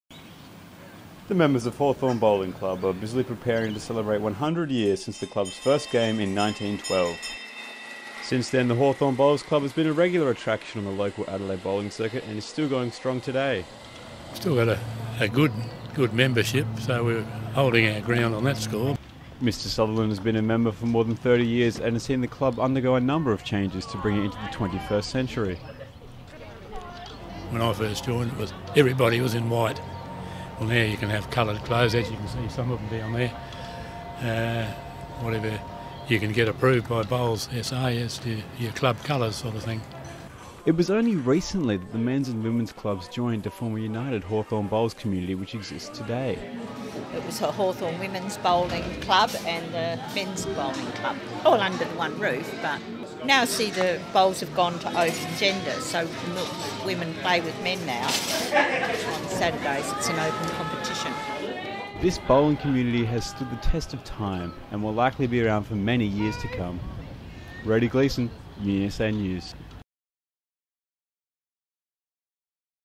Hawthorn Bowls Centenary
Hawthorn Bowls Club is busy preparing for its biggest event of the year as members get ready to celebrate the club's centenary this month.

Rhody Gleeson drops in on the festivities and investigates how the club has changed over the years.